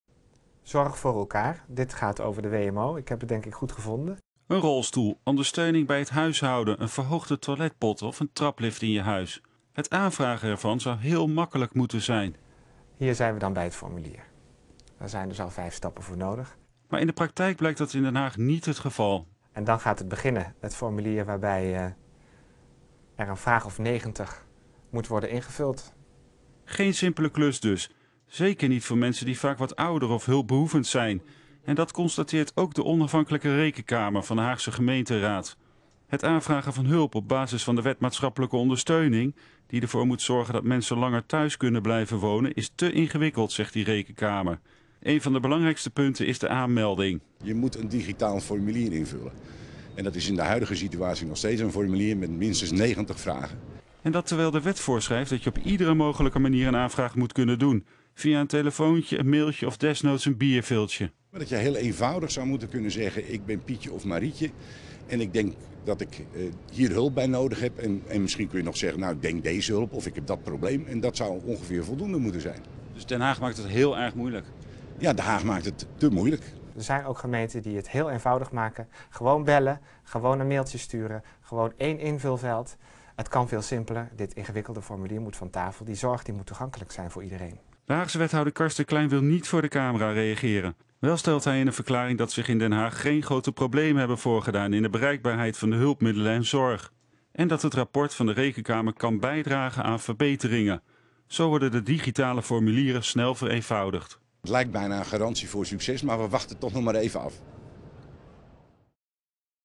Gemeente Den Haag overtreedt regels rond Wmo
De gemeente Den Haag maakt het aanvragen van zorg en ondersteuning te
moeilijk. Dat concludeert de Haagse Rekenkamer na onderzoek naar de
uitvoering van de Wet maatschappelijke ondersteuning.